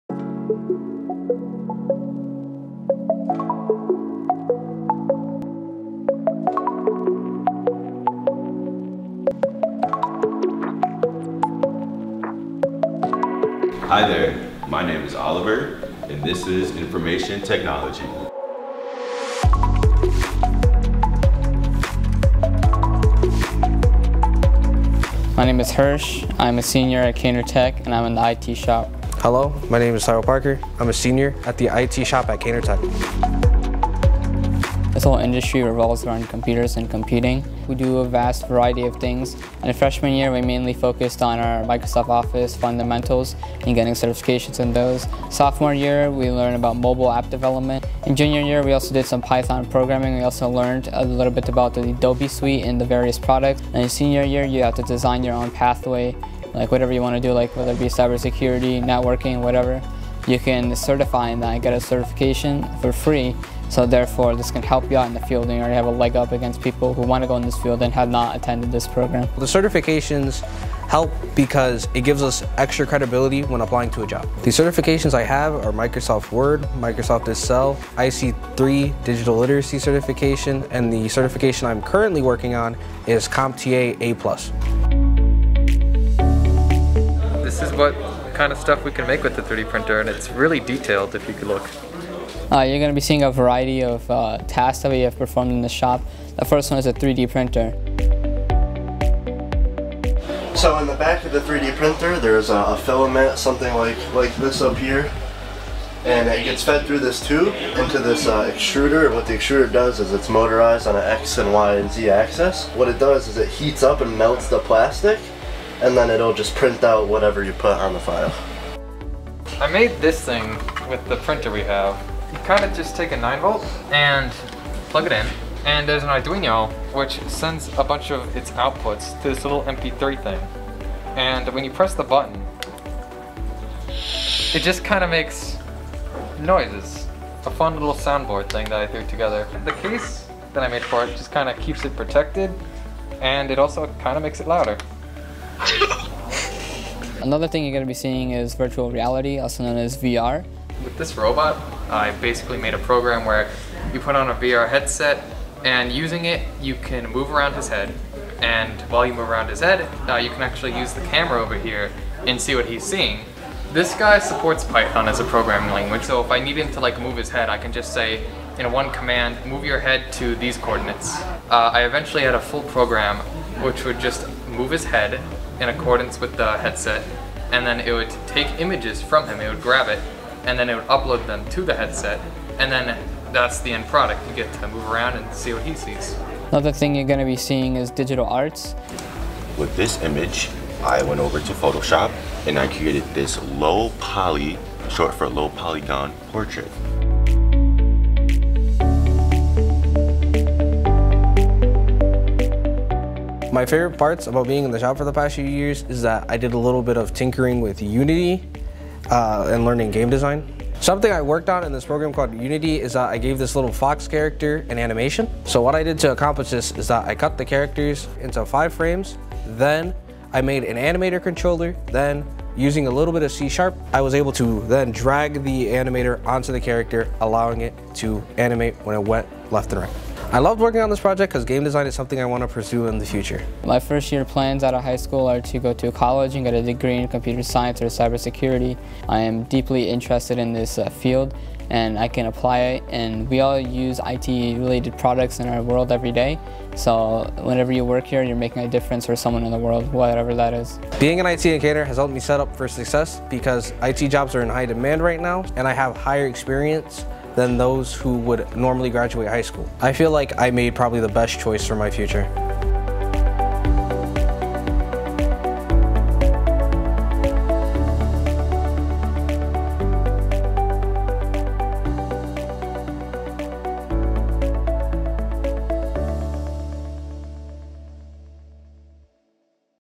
Information Technology at Connecticut's Technical High Schools; Kaynor Tech High School
Students deciding to enter the field of Information Technology are introduced to basic concepts of computer hardware and software. Basic computer programming, school-to-work and job readiness skills are also introduced. Following general study in IT, students select a pathway from among 6 options and develop an individual learning plan in cooperation with their teacher.  As students work independently toward employment or higher education in their area of focus, they prepare for and take certification exams related to their pathway.  IT pathways include:

Management Information Systems
Interactive Web Content and Digital Media
Computer Science and Programming
Computer Networking and CyberSecurity
Hardware Troubleshooting
Web Development